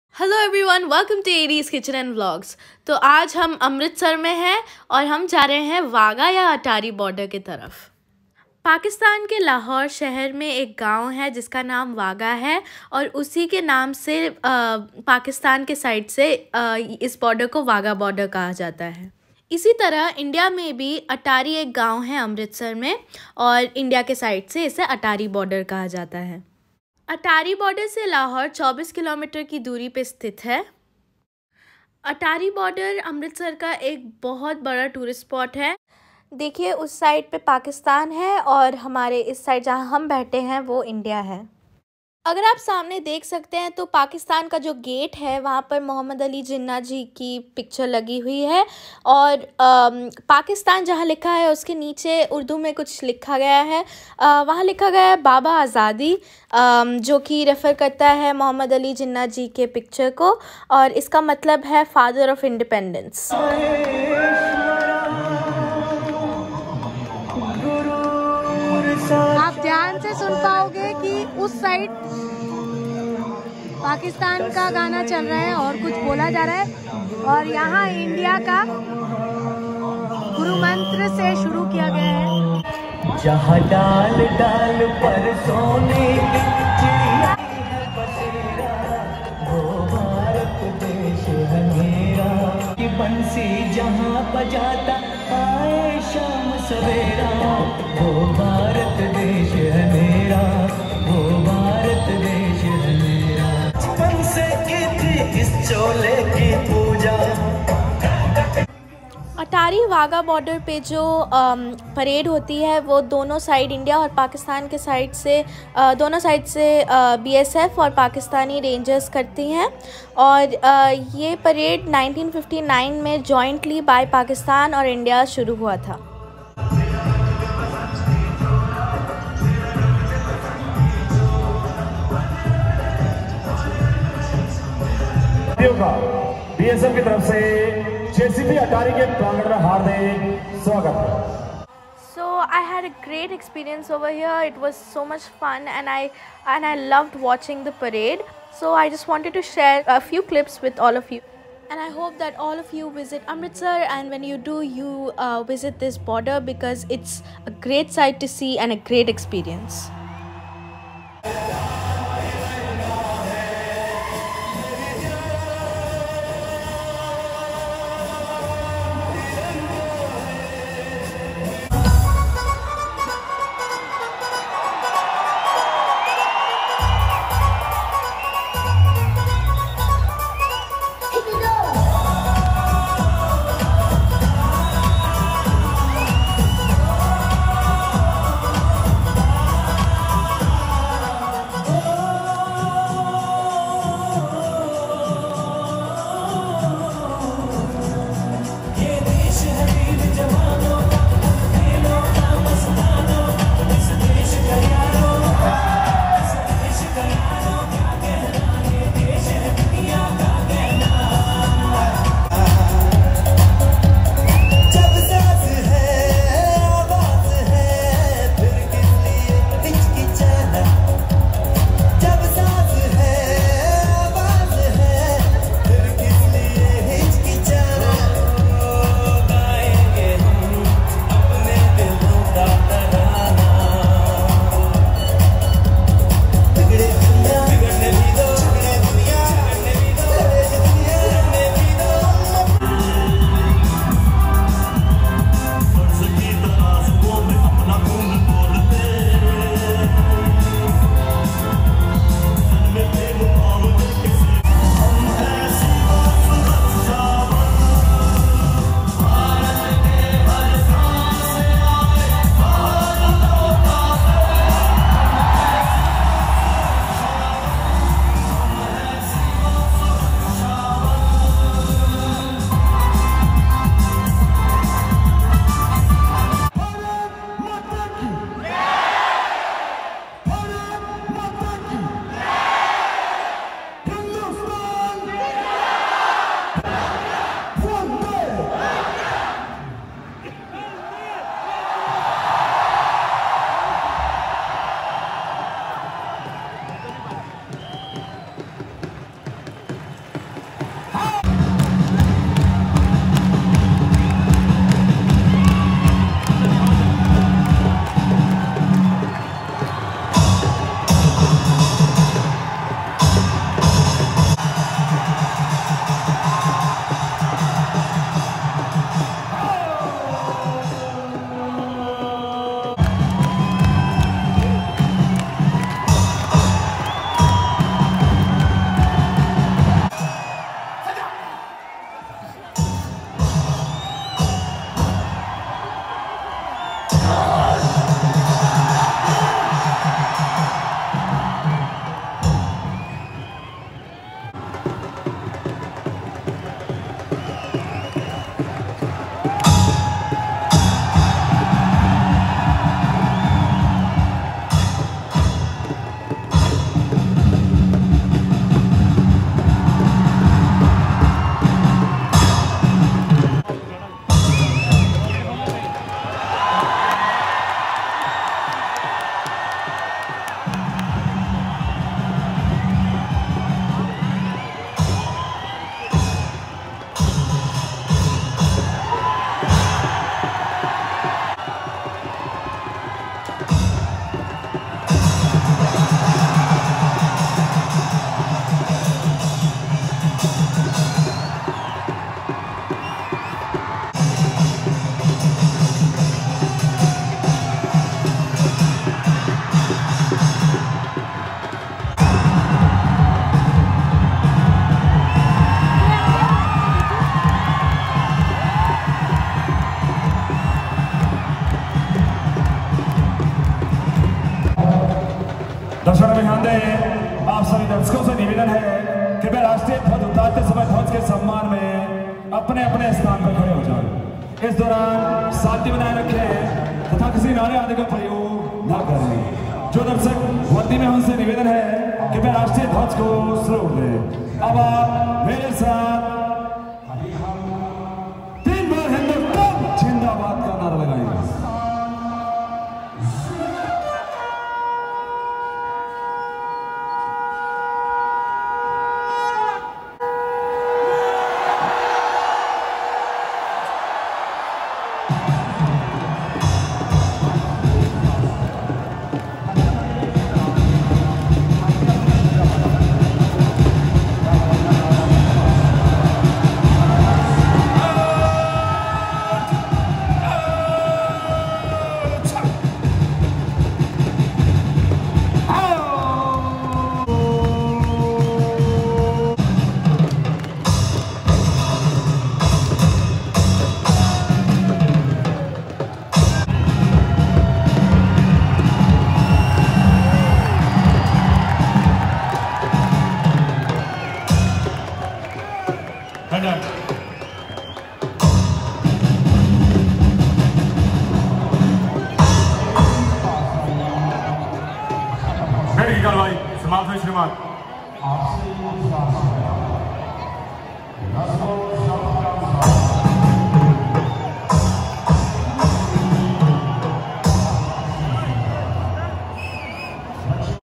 Attari Border || India-Pakistan Border || BSF Parade
It was a great experience in the Wagah-Atari border which is a India-Pakistan border in Amritsar from India side and parades that happen here are led by Indian BSF and Pakistani Rangers.